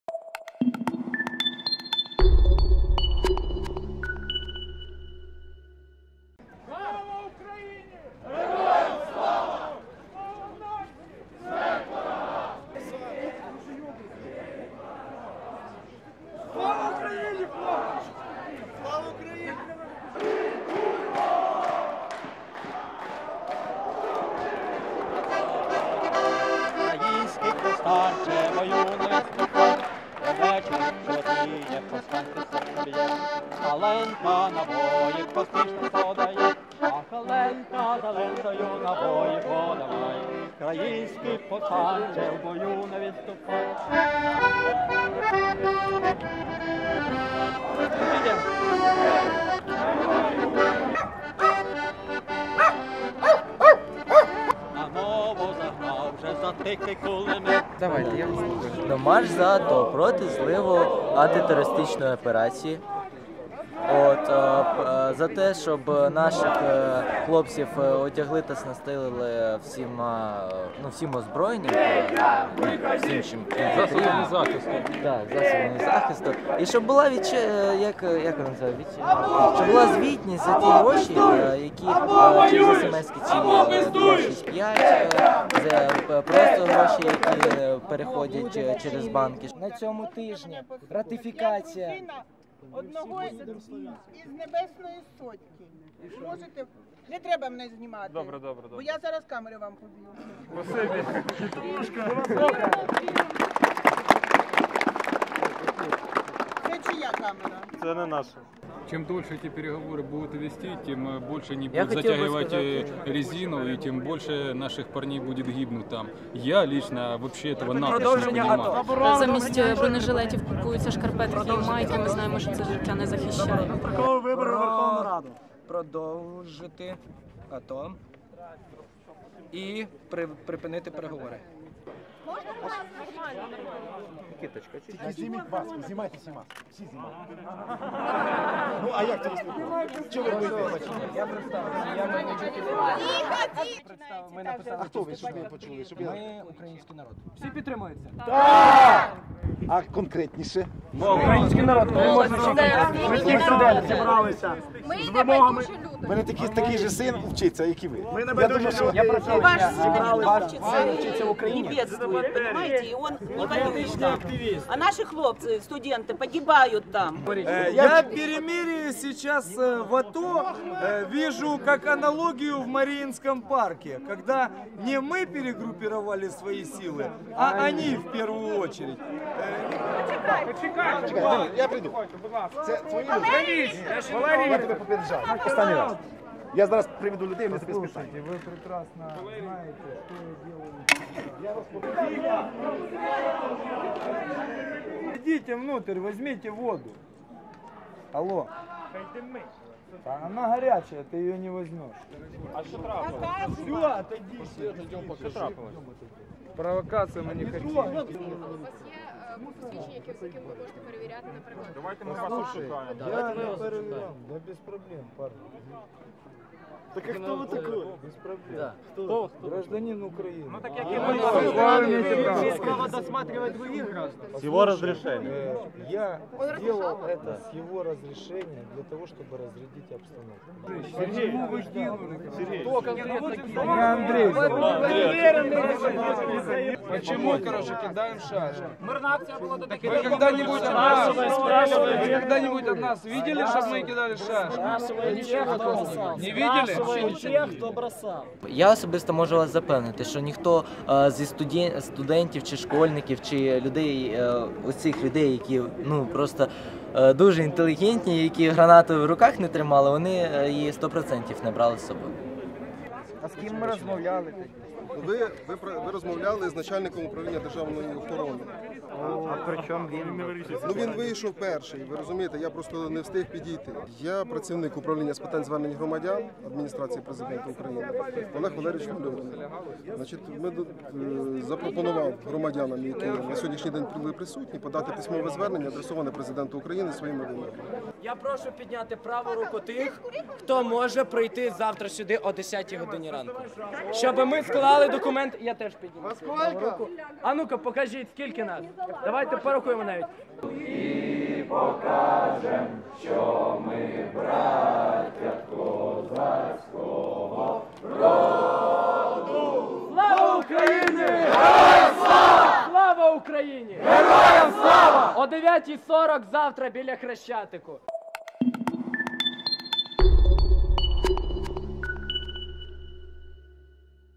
Мирна хода проти "зливу" АТО
Студенти, школярі та просто активісти прошлися Києвом з мирною ходою проти "зливу" АТО.

"Замість бронежилетів купляються шкарпетки і майки - ми знаємо, що це життя не захищає" - каже одна з активісток.

З Контрактової площі, через Майдан, протестувальники прийшли під Адміністрацію Президента і кликали ґаранта конституції до себе.

Під час спілкування з начальником управління державної служби безпеки України, хтось з активістів кинув у бік входу в АП димову шашку і камінь. Зав'язалася декількохвилинна сутичка, яка сама по собі вирішилася. Ніхто не постраждав.

Активістам порадили прийти завтра з офіційним листом на ім'я Президента, де вони змогли б викласти усі свої вимоги Мітингувальники погодилися і розійшлися. Частина з них пішли в бік Верховної ради України.

/ Нікіта Мєкєнзін